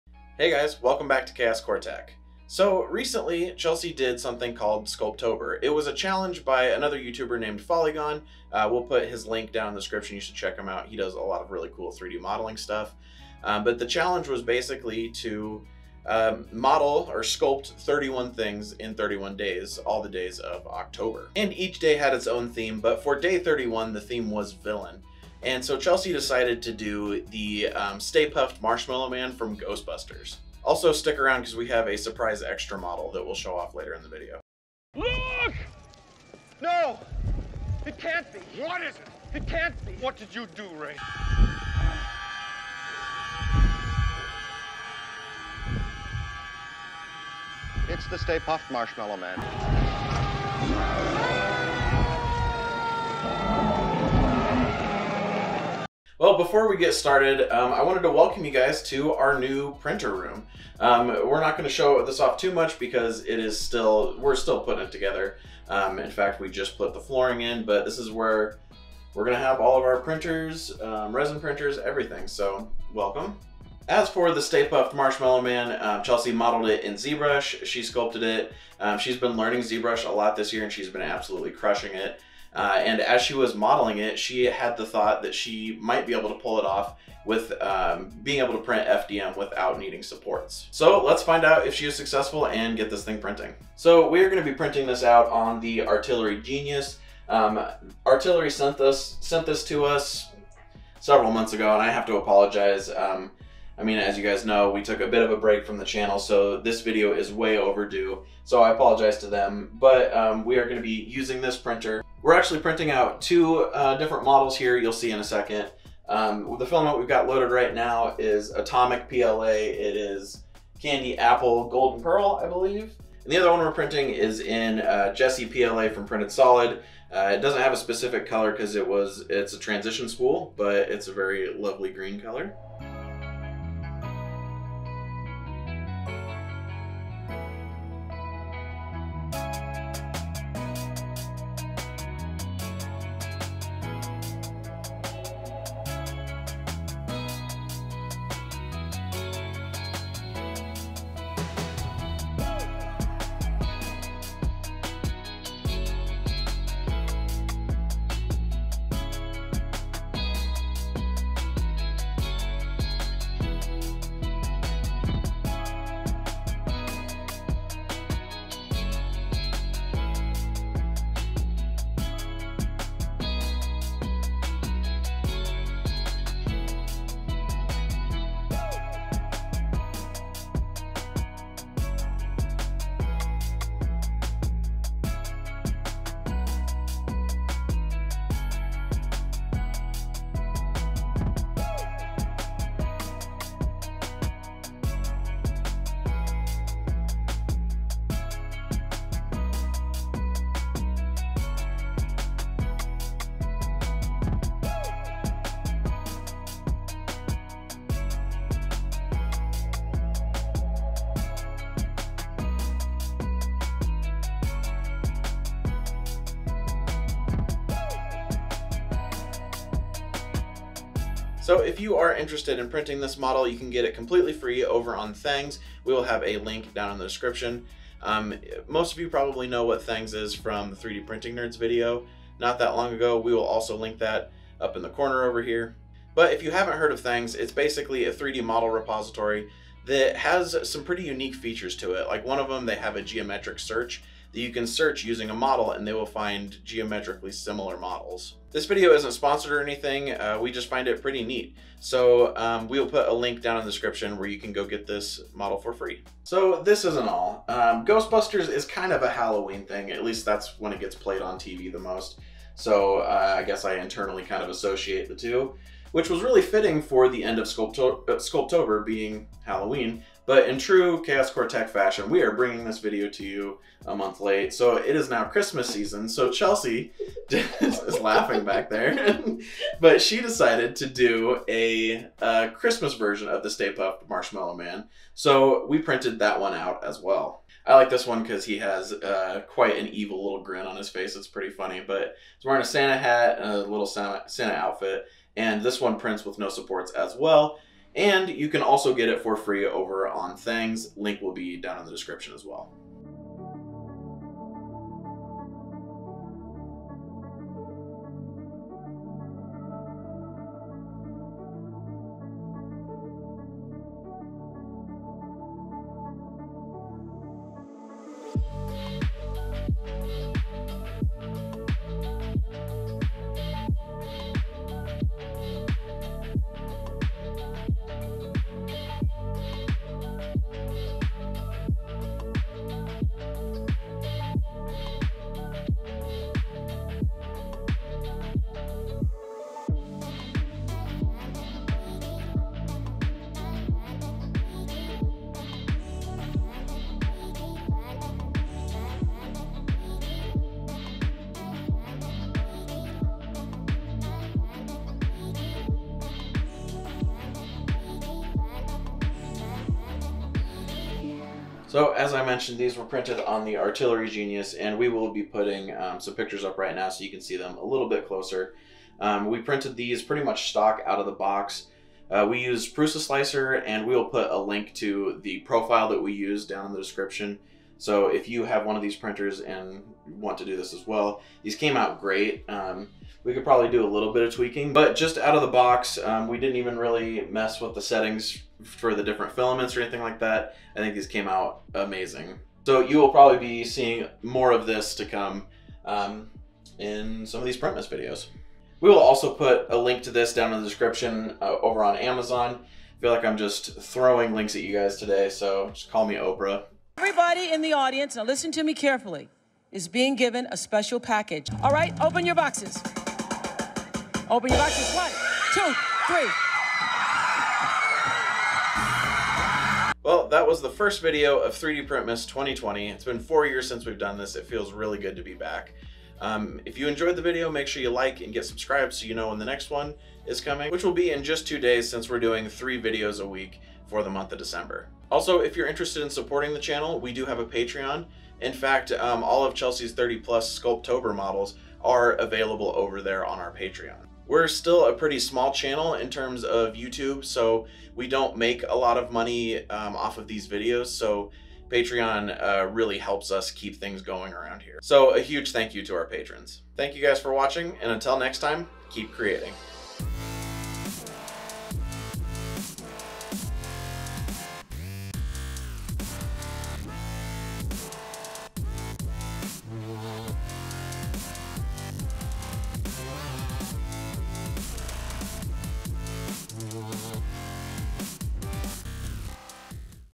3D Printed Stay Puft Marshmallow Man [Ghostbusters] - Prints With No Supports!
For Day 31 of Sculptober, Chelsey 3D Modeled the Stay Puft Marshmallow Man from Ghostbusters. She modeled it in ZBrush and managed to make it print with no need for supports!

OTHER THINGS WE RECOMMEND ON AMAZON (aff):